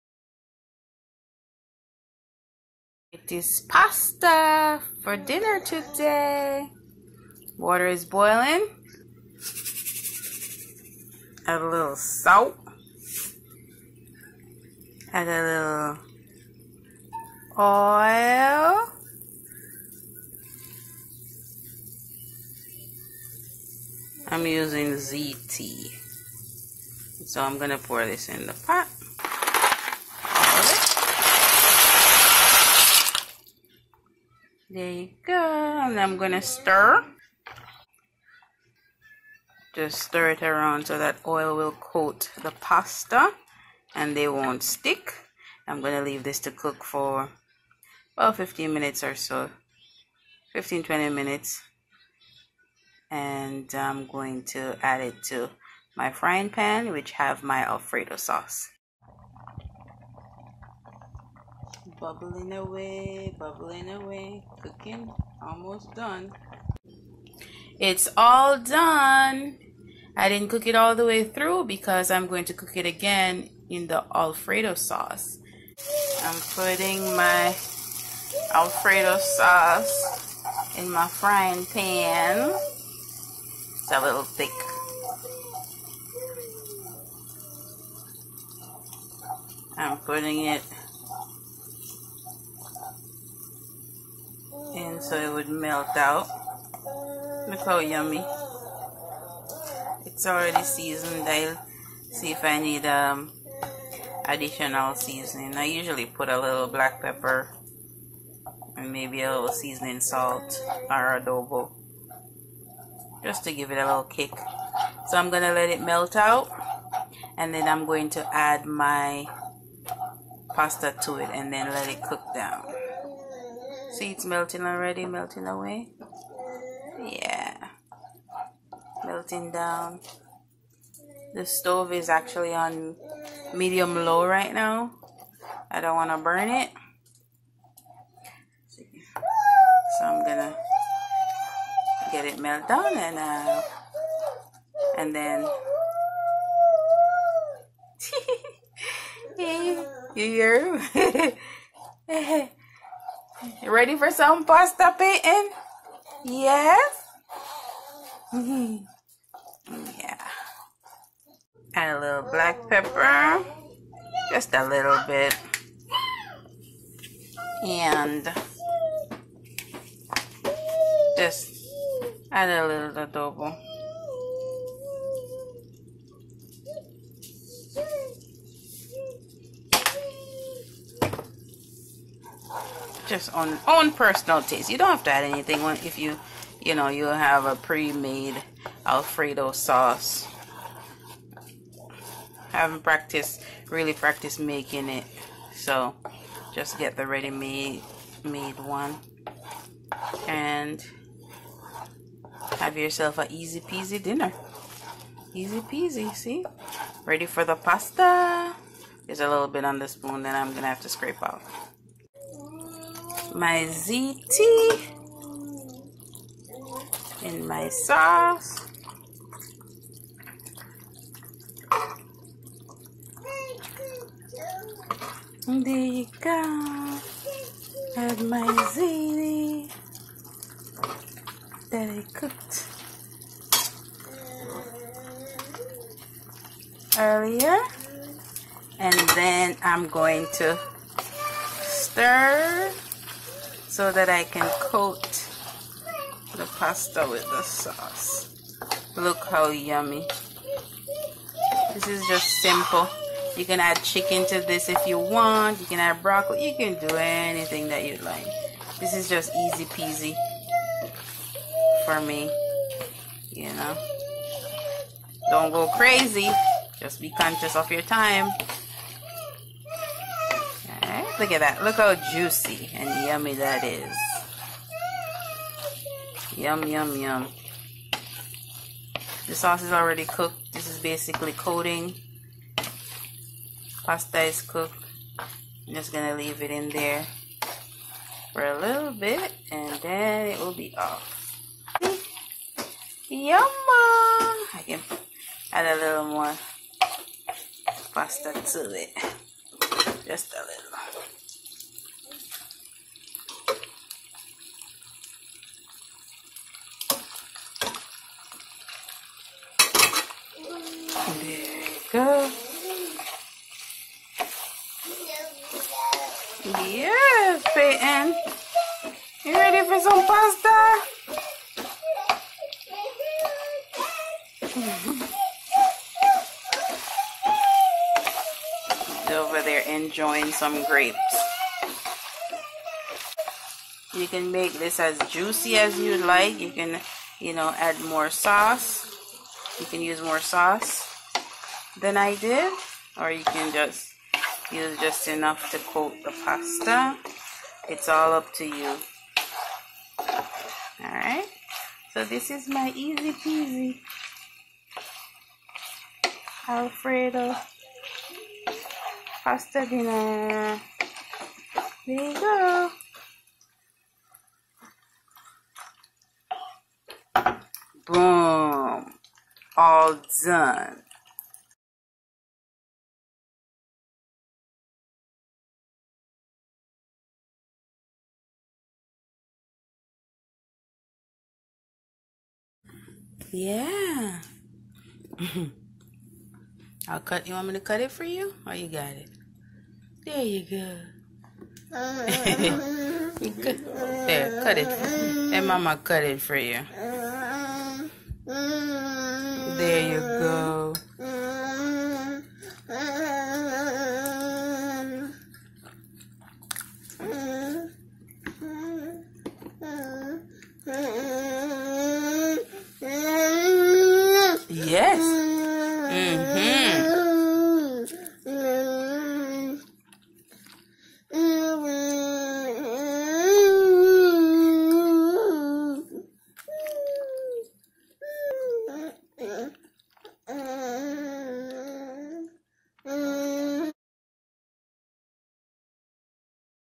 Simple affordable Alfredo Pasta for dinner under $2.00| Cooking alfredo pasta. Toddler Approved
Simple affordable Alfredo Pasta for dinner under $2.00.
Pre-made Alfredo Sauce with Ziti.

Price of Dinner:
pre made Alfredo sauce $.99 sale
Ziti $.88 sale

Cook ziti for 15/20 depends on how you like your pasta.
While Pasta cooks heat Alfredo sauce and add anything you like to it, like chicken broccoli.
Mix cooked Pasta into Alfredo sauce, and stir to mix well, and coat pasta.
Then serve.